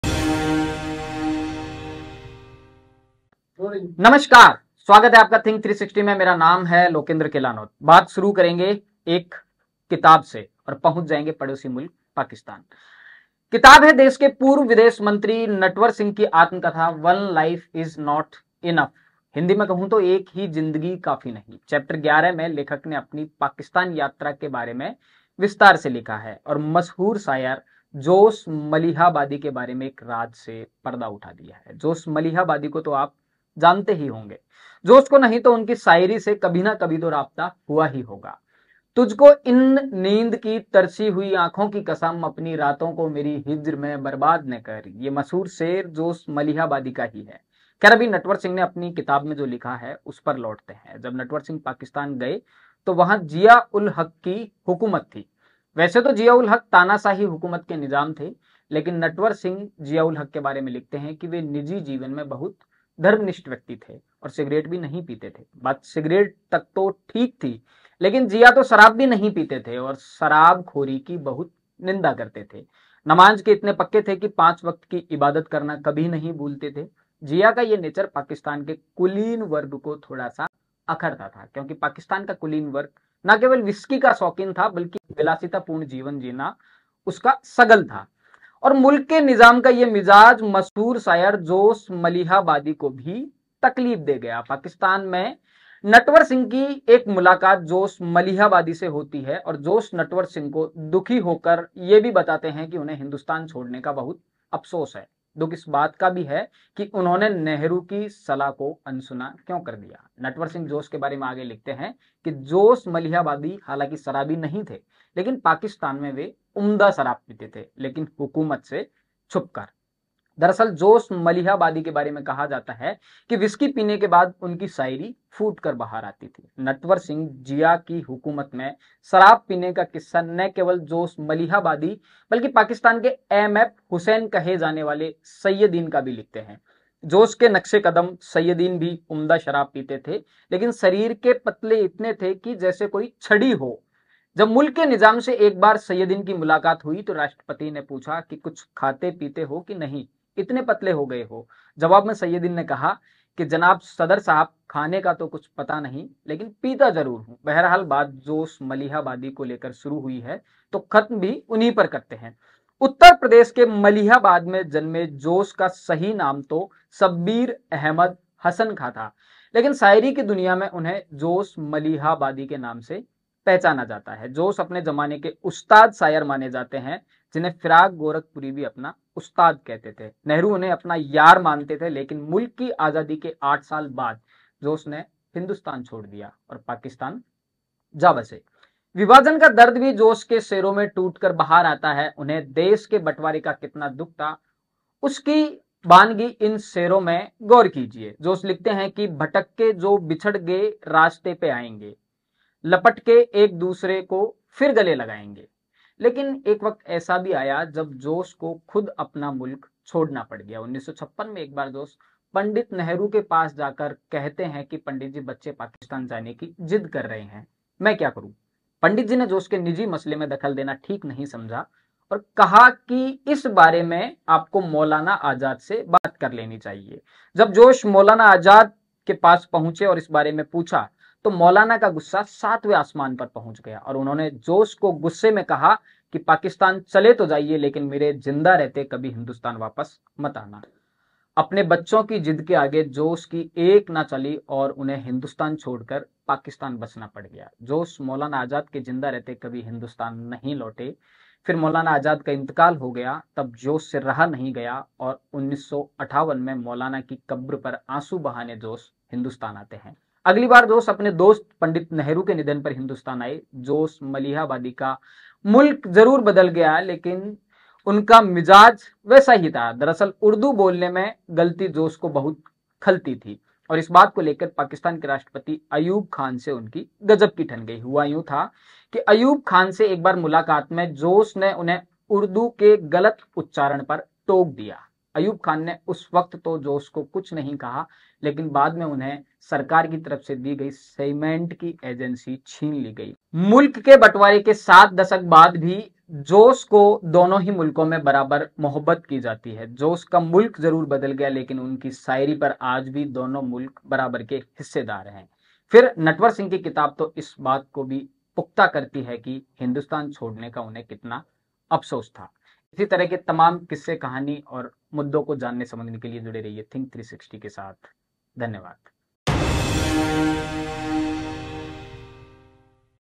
Maulana Azad ने Josh Mlihabadi को कहा कि मेरे ज़िंदा रहते कभी Hindustan मत आना
बात शुरू करेंगे एक किताब से और पहुँच जाएंगे पड़ोसी मुल्क पाकिस्तान. किताब है देश के पूर्व विदेश मंत्री नटवर सिंह की आत्मकथा वन लाइफ इज नॉट इनफ हिंदी में कहूँ तो एक ही ज़िंदगी काफी नहीं.
चैप्टर ग्यारह में लेखक ने अपनी पाकिस्तान यात्रा के बारे में विस्तार से लिखा है. और मशहूर शायर जोश मलीहाबादी के बारे में एक राज से पर्दा उठा दिया है. जोश मलीहाबादी को तो आप जानते ही होंगे. जोश को नहीं तो उनकी शायरी से तो कभी ना कभी राब्ता हुआ ही होगा.
तुझको इन नींद की तरसी हुई आंखों की कसम अपनी रातों को मेरी हिज्र में बरबाद न कर
ये मशहूर शेर जोश मलीहाबादी का ही है.
खैर अभी नटवर सिंह ने अपनी किताब में जो लिखा है उस पर लौटते है. जब नटवर सिंह पाकिस्तान गए तो वहां जिया उल हक़ की हुकूमत थी. वैसे तो जिया उल हक़ तानाशाही हुकूमत के निजाम थे लेकिन नटवर सिंह जिया के बारे में लिखते है कि वे निजी जीवन में बहुत धर्मनिस्ट व्यक्ति थे और सिगरेट भी नहीं पीते थे. बात सिगरेट तक तो ठीक थी लेकिन जिया उल हक़ ना केवल शराब पीते थे बल्कि शराबखोरी की बहुत निंदा भी करते थे. नमाज के इतने पक्के थे कि पांच वक्त की इबादत करना नहीं भूलते थे.